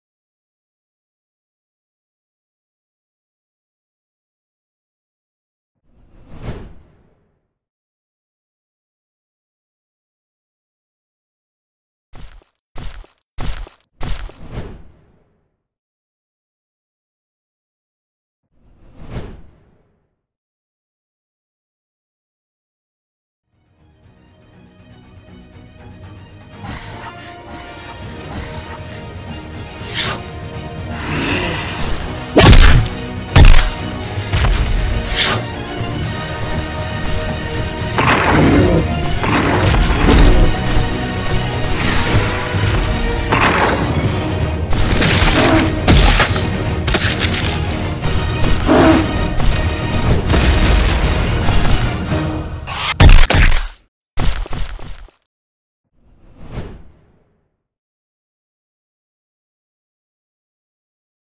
Battle League: 1-3 (Prenocephale vs Guanlong)
Hi my dear freakysaurs! I hope you enjoyed the video!

Feel free to download and use my stks, but you must ask for permission in order to use them on public projects.

Next battle coming soon...

Prenocephale and Guanlong made by me, based on Cisiopurple's Deviantart designs.
Psittacosaurus was made public by a youtuber that does not exist anymore (I don't know who they were)